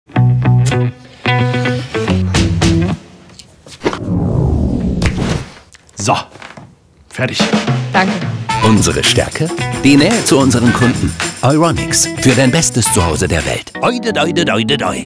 Bester Service // TV - EURONICS Imagespot mit Almuth Schult
Als Europas größter Elektronikhändler engagieren wir uns für den europäischen Spitzenfrauenfußball.

In der UEFA Women’s Champions League spielen die großen Namen des europäischen Vereinsfußballs: u.a. Real Madrid, CF Barcelona, Bayern München, Chelsea London. Die Europameisterschaft in England wird das topp Sport-Highlight in diesem Sommer. Das zeigt: Frauenfußball ist oben angekommen.

Aber auch ein zweites Thema hat uns motiviert: Gleichberechtigung und Chancengleichheit für Europas Frauen. Unser Engagement gehen wir zusammen mit der bekanntesten deutschen Fußballerin an: der Nationaltorhüterin Almuth Schult.

Almuth Schult gehört zu den erfolgreichsten deutschen Fussballerinnen des letzten Jahrzehnts: Olympiasiegern, Europameisterin, Champions League-Siegerin, mehrmalige Deutsche Meisterin sowie Pokalsiegerin und nicht zuletzt auch Welttorhüterin. Einem breiteren Publikum wurde sie als ARD-Fußballexpertin während der Männer-Fußball-EM 2021 bekannt. Sie ist Teil der Initiative „Fußball kann mehr“ für mehr Teilhabe und Diversität im deutschen Fußball.